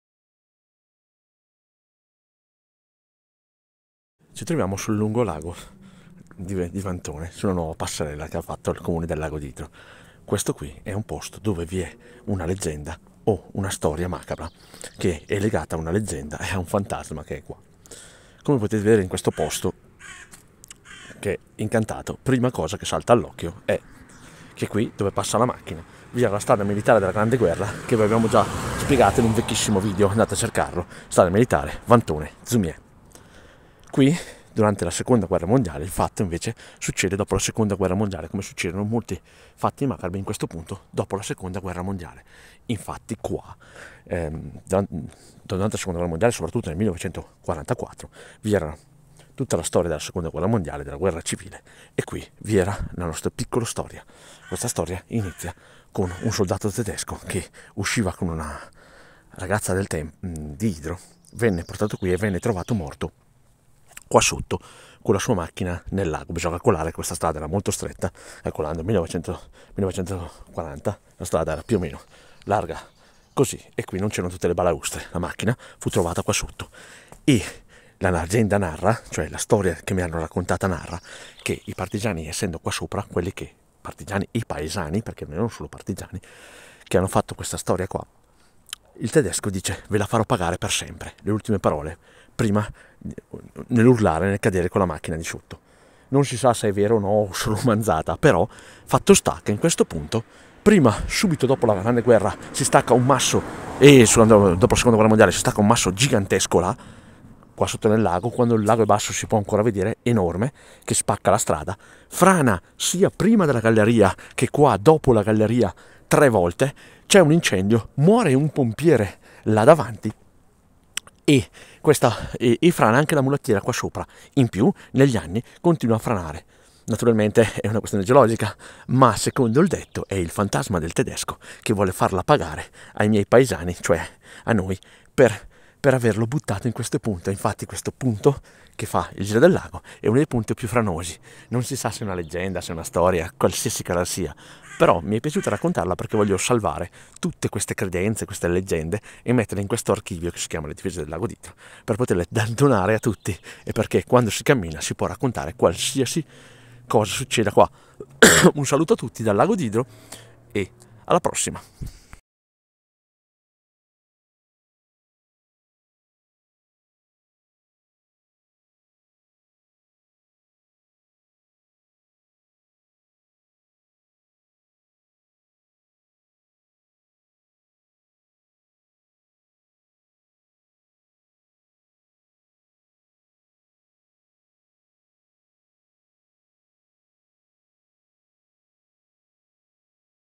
il fantasma del tedesco sul lago d'Idro (il suo anatema)
un racconto della seconda guerra mondiale tragico accaduto qui . sul lago e la storia mista leggenda del anatema del tedesco